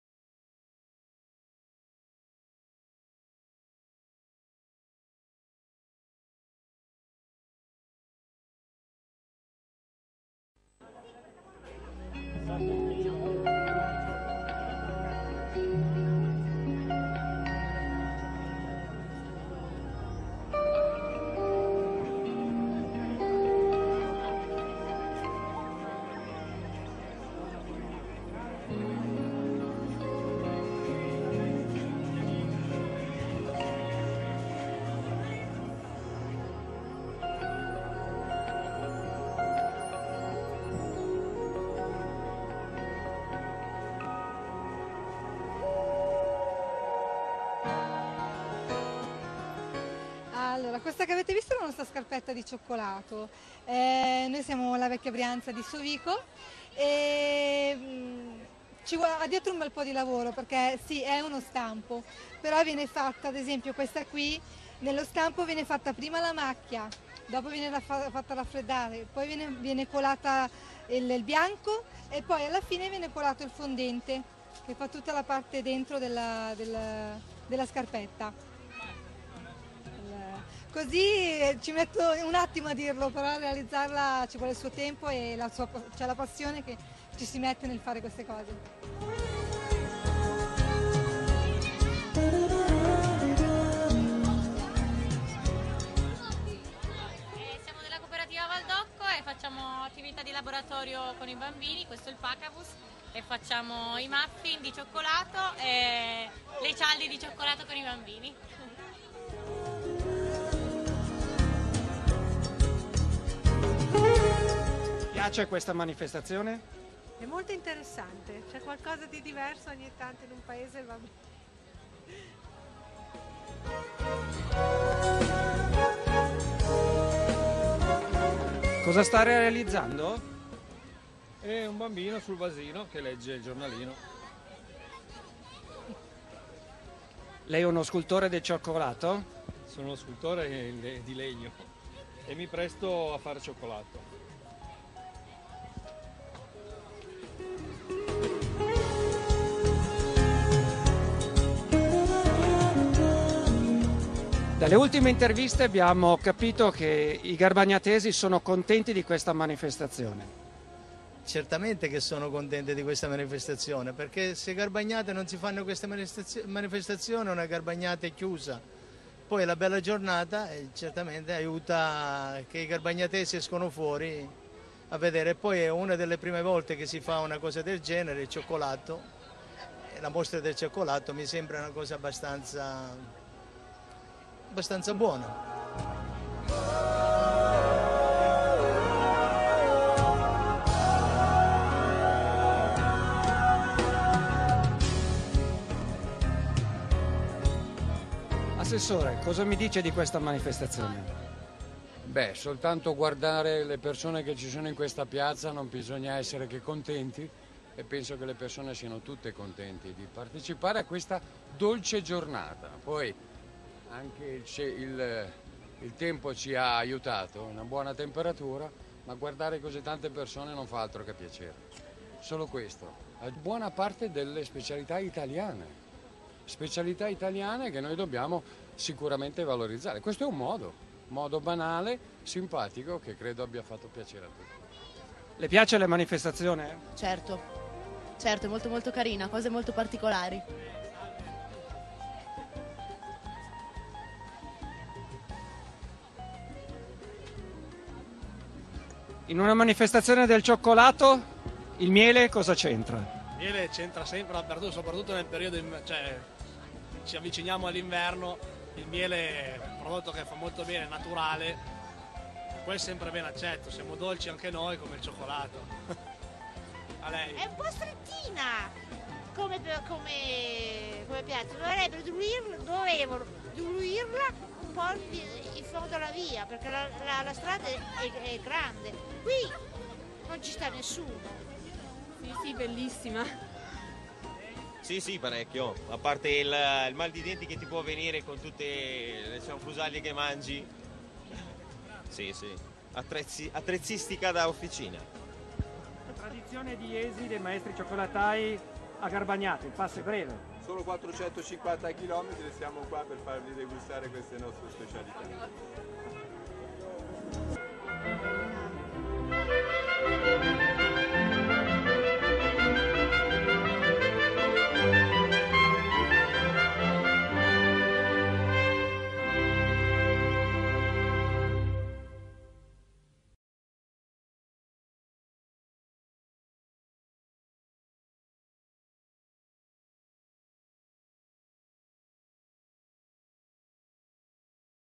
Grande Festa del Cioccolato
È l´evento più dolce dell´anno: domenica 13 novembre, dalle 10:00 alle 19:00, Piazza della Croce ospita La Grande Festa Del Cioccolato, la novità più squisita di questo 2011.